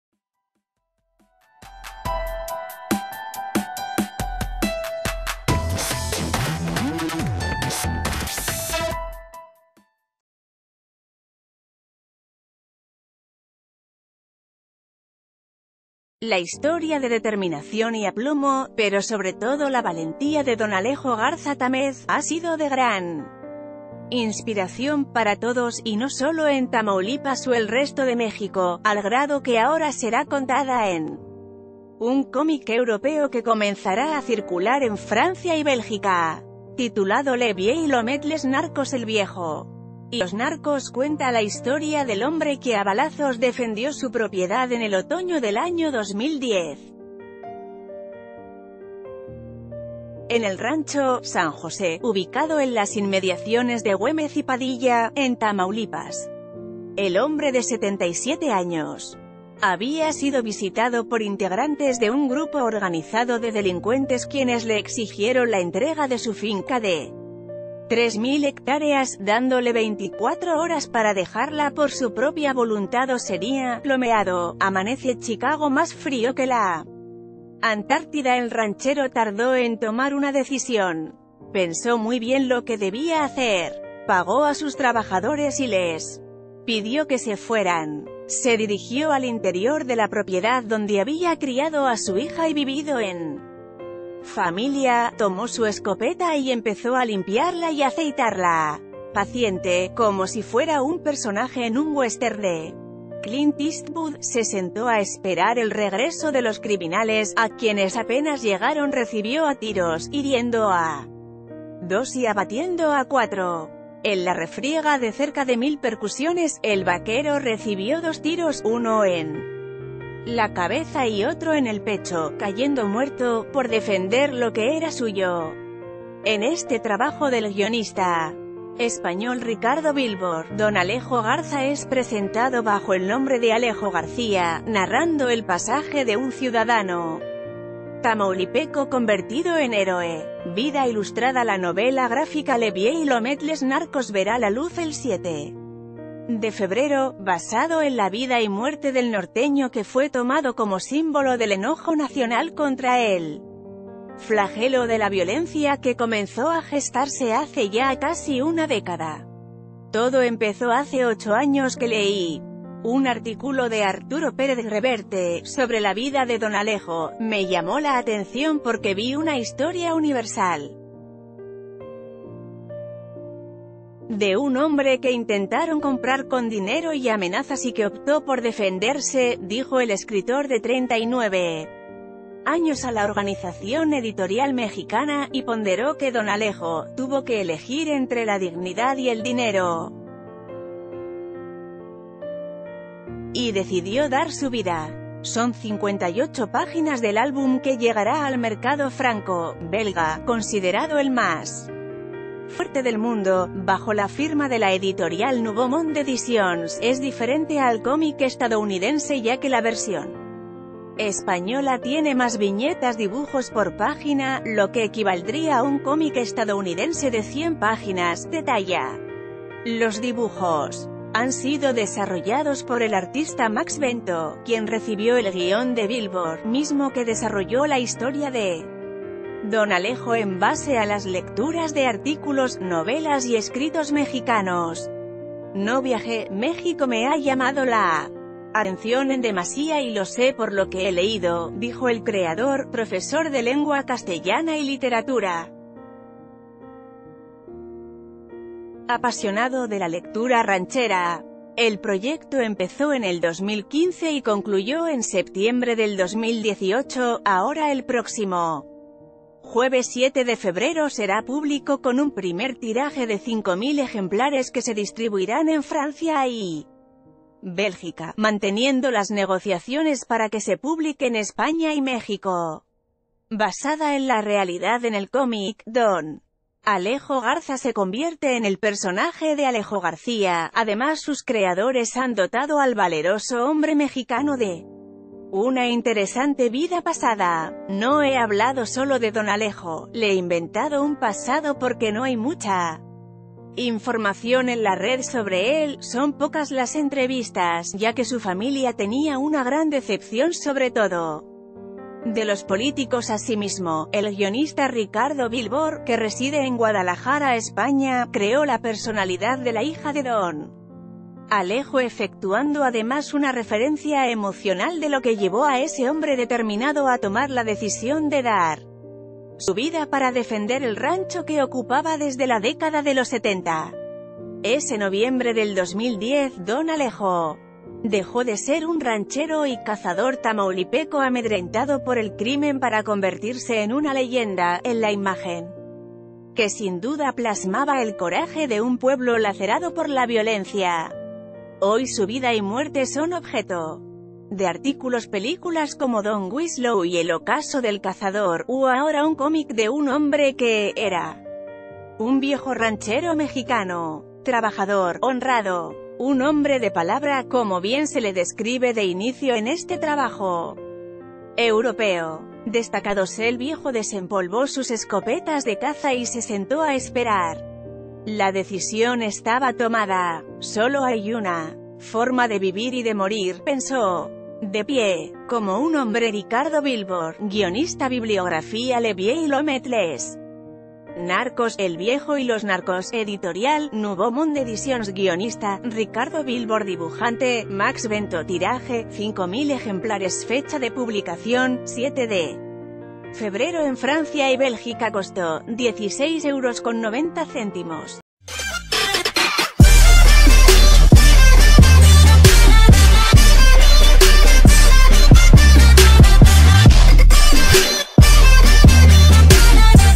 La historia de Don Alejo, el héroe tamaulipeco, llega al comic
La historia de determinación y aplomo, pero sobre todo la valentía de don Alejo Garza Tamez, ha sido de gran inspiración para todos, y no sólo en Tamaulipas o el resto de México, al grado que ahora será contada en un comic europeo que comenzará a circular en Francia y Bélgica. Titulado Le vieil homm...